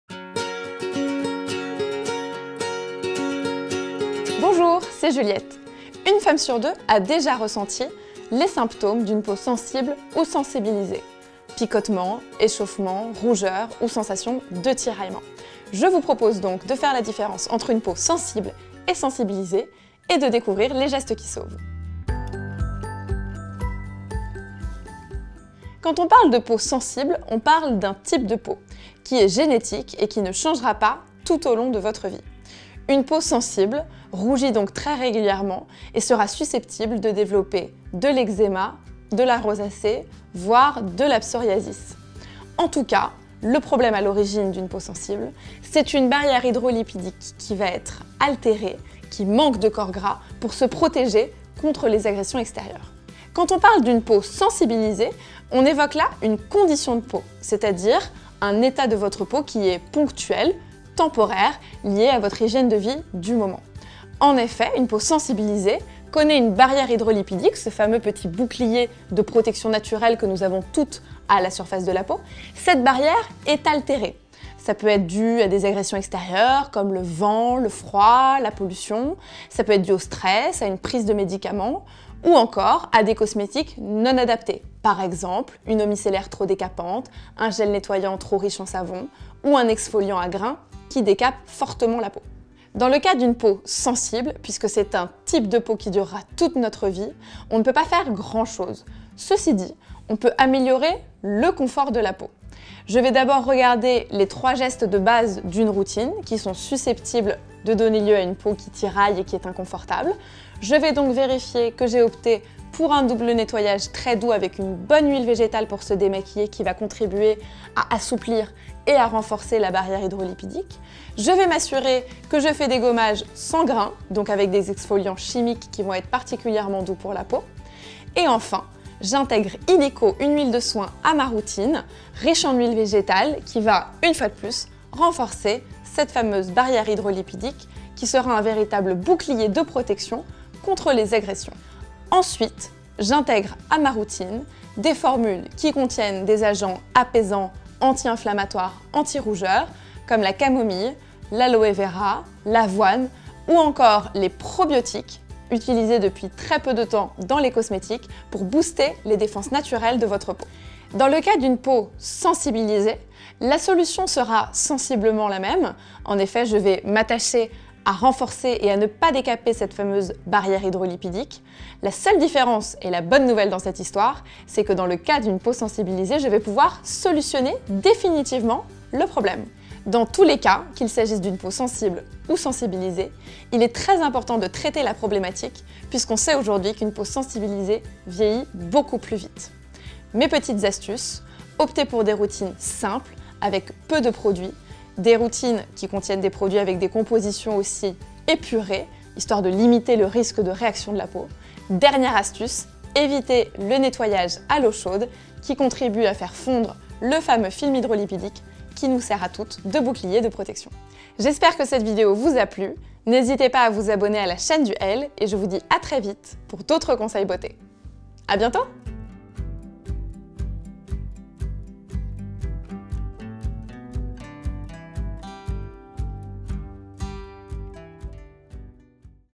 Comment prendre soin d'une peau sensible ?┃ELLE Skincare
Pas facile de trouver une routine de soins adaptée quand on a la peau sensible... Notre peau est-elle sensible ou simplement sensibilisée ? Quels soins lui apporter ? Comment lutter contre les rougeurs ? Avec Juliette d'Oh My Cream, on décortique le problème.

REMERCIEMENTS

Production : LED